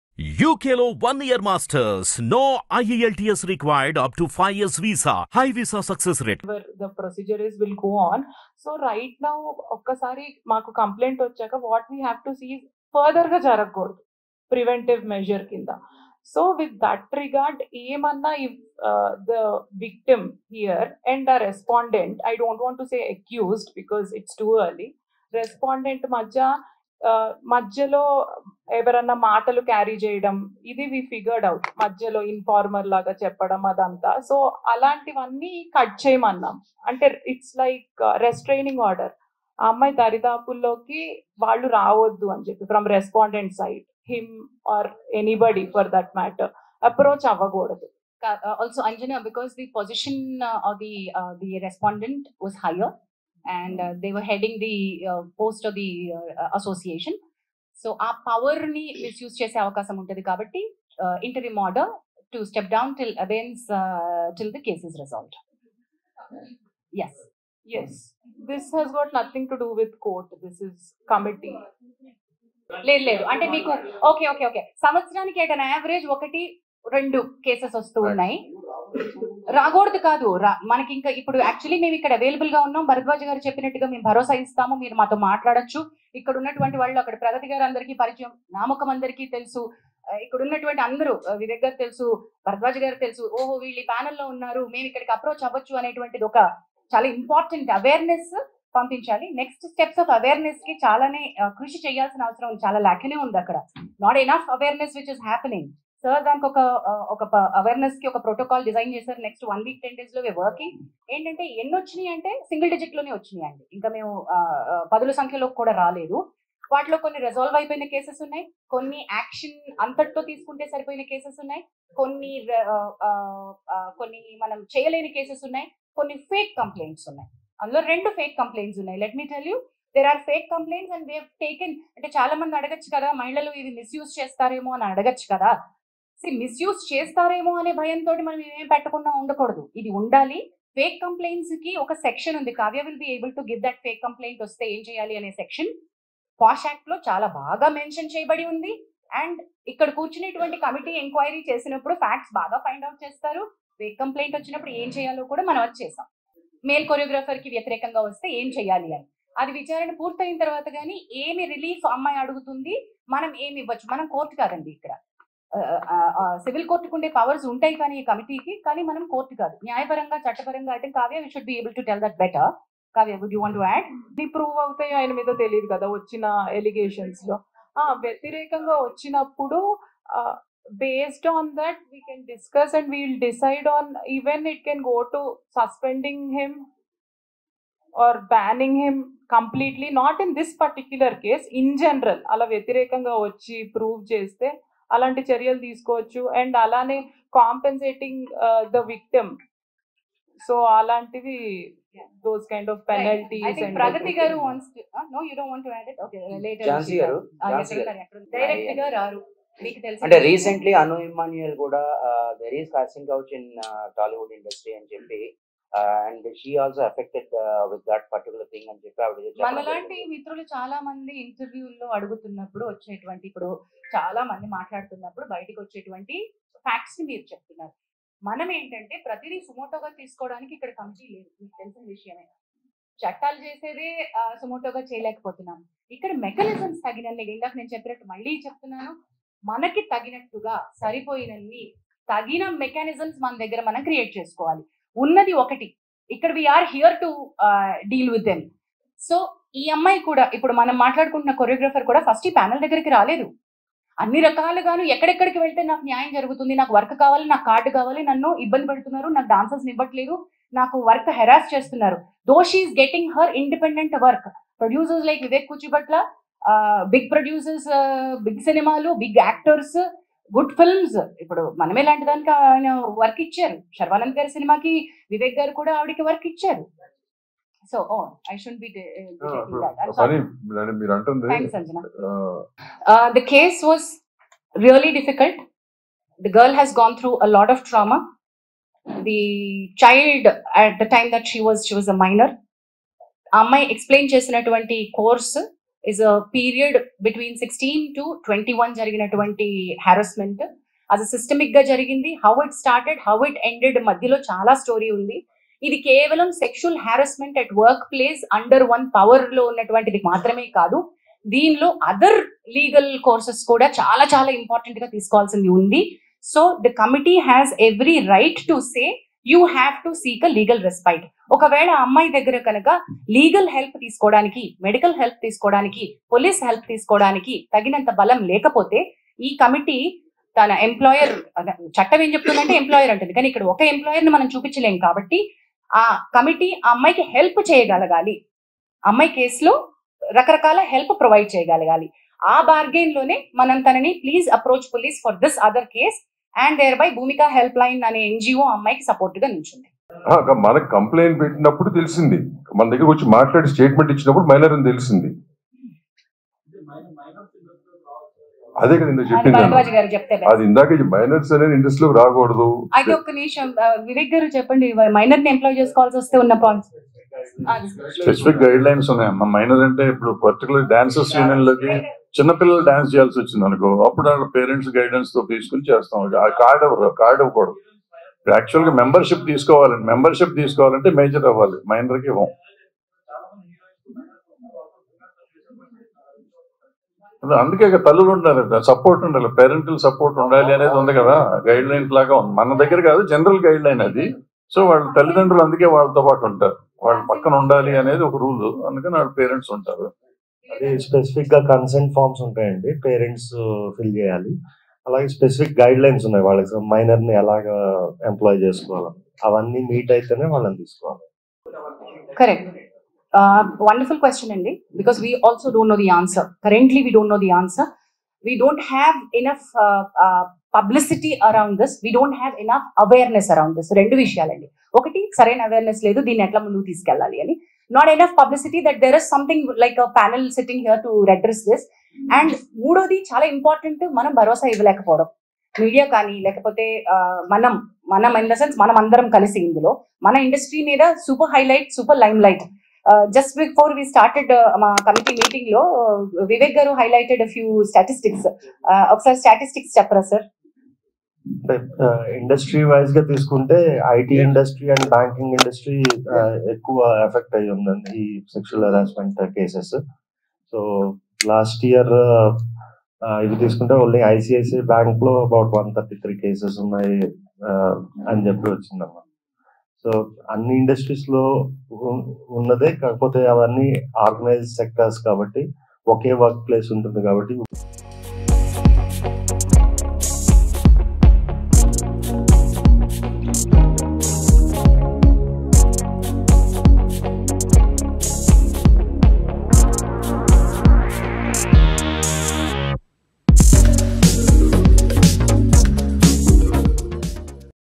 జానీ మాస్టర్ ఇండస్ట్రీ నుంచి బ్యాన్ ..? - Film Chamber Members About Jani Master@HITTVOFFICIAL
Watch : జానీ మాస్టర్ ఇండస్ట్రీ నుంచి బ్యాన్ ..? - Film Chamber Members About Jani Master @HITTVOFFICIAL